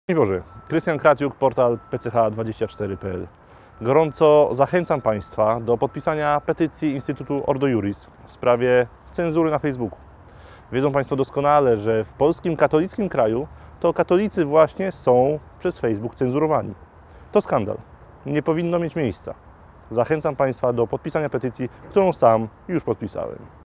Krystian Kriatiuk o cenzurze Katolików w Mediach Społecznościowych
Również Krystian Kratiuk poparł petycję Ordo Iuris  ws. wolności słowa w Mediach Społecznościowych. Zachęcamy do udostępniania nagrania z redaktorem naczelnym PCH24.pl  i poparcia petycji.
Bronimy wolności słowa w Internecie.